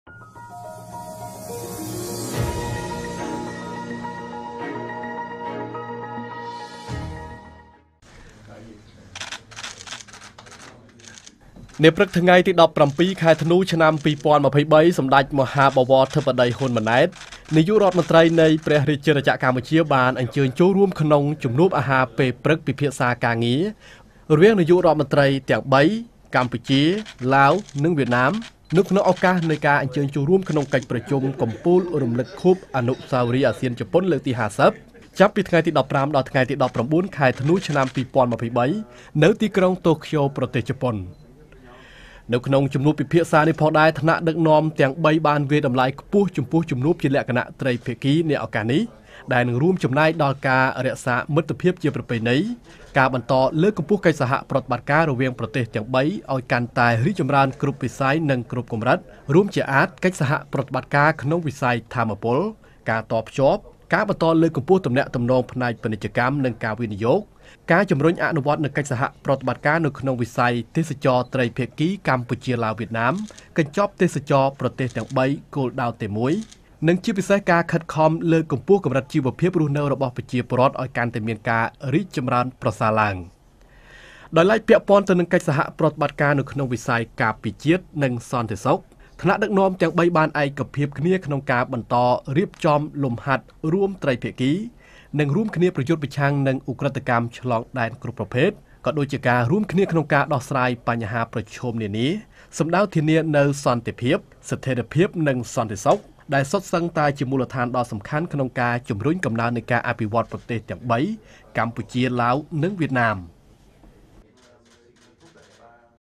សម្តេចធិបតី​ អញ្ជើញចូលរួមពិសារអាហារការងារពេលព្រឹក ជានាយករដ្ឋមន្ត្រីឡាវ​ និងវៀតណាម
វីដេអូ៖​សម្តេចមហាបវរធិបតី ហ៊ុន ម៉ាណែត នាយករដ្ឋមន្ត្រី នៃព្រះរាជាណាចក្រកម្ពុជា អញ្ជើញចូលរួមពិសារអាហារការងារពេលព្រឹក ជាមួយ ឯកឧត្តម សនសៃ ស៊ីផាន់ដន (Sonexay Siphandone) នាយករដ្ឋមន្រ្តី នៃសាធារណរដ្ឋប្រជាមានិតប្រជាធិបតេយ្យឡាវ និង ឯកឧត្តម ផាម មិញជិញ (Pham Minh Chinh) នាយករដ្ឋមន្រ្តី នៃសាធារណរដ្ឋសង្គមនិយមវៀតណាម

ទីក្រុងតូក្យូ ប្រទេសជប៉ុន នៅព្រឹកថ្ងៃអាទិត្យ ៥កើត ខែមិគសិរ ឆ្នាំថោះ បញ្ចស័ក ព.ស. ២៥៦៧​ត្រូវនឹង ថ្ងៃទី១៧​ ខែធ្នូ ឆ្នាំ២០២៣